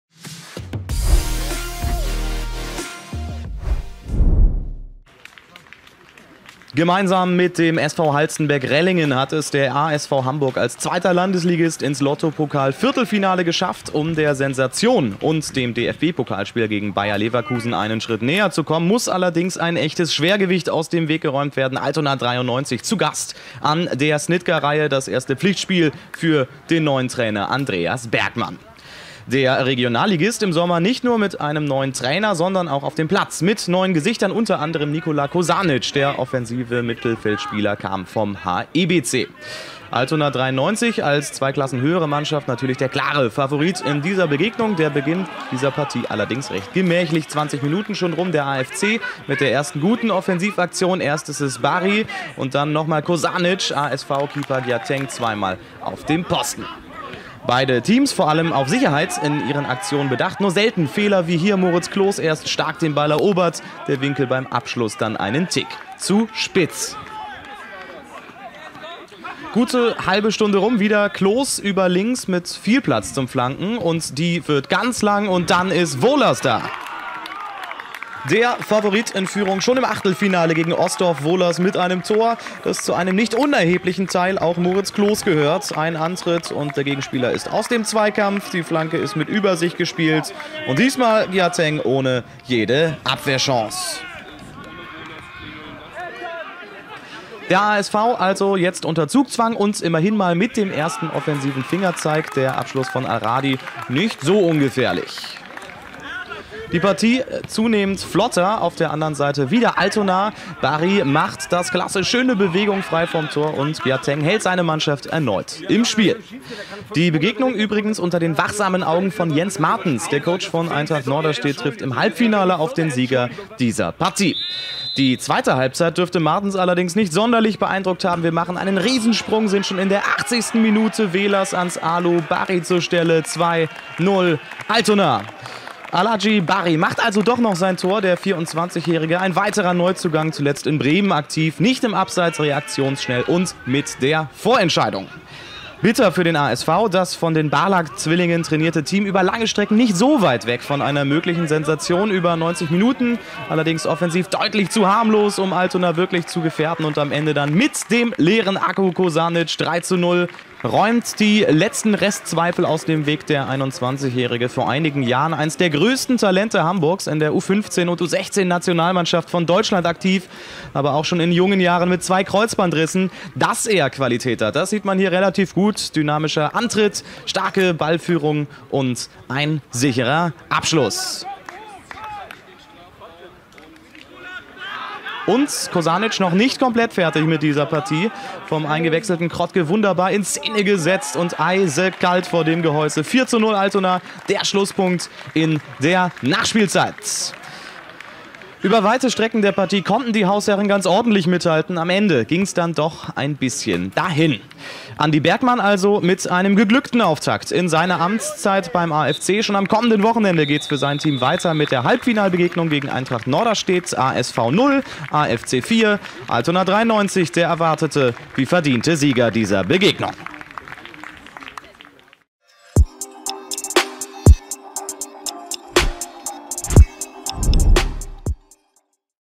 Kosanic-Show bringt Altona ins Halbfinale | ASV Hamburg - Altona 93 (Viertelfinale, Pokal)
Andreas Bergmann mit geglückter Premiere als AFC-Coach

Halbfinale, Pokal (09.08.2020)

Tore:
0:1 Ole Wohlers
0:2 Aladji Barrie
0:3 Nikola Kosanic
0:4 Nikola Kosanic

Aufstellung ASV Hamburg:
30 Nick Obeng Gyateng, 2 Mike Appiah, 3 Benino Russo, 5 Vedat Barlak, 6 Ercan Ileri, 7 Alesson Fernandes Arguelho, 9 Timo Aschenbrenner, 10 Abdul-Nafe Farahi, 11 Rathwan Al Radi, 14 Deniz Herber, 15 Youcef Madadi

Ersatzbank:
39 Michael Albot, 8 Argetim Kaba, 16 Nazar Ahmad Masharqi, 17 Abdul Samey Ludin, 20 Alan Jacek Szymborksi, 22 Maximilian Andre Kochsiek, 91 Malang Lamin Jawla

Trainer:
Mekan Barlak, Devran Barlak

Aufstellung Altona 93:
Tobias Braun, Ole Wohlers, Leon Mundhenk, Eudel Silva Monteiro, Niklas Siebert, Moritz Kloß, Nikola Kosanic, Aladji Barrie, Emre Cem Töremis, Hendrik Bombek, Marco Heskamp

Ersatzbank:
30 Nathanael Obeng Sallah, Jasper Brinkmann, Valentin Brandis, Kevin Krottke

Trainer:
Andreas Bergmann

Produktion: Frederik Sehner
Kommentar: Tim Hemmrich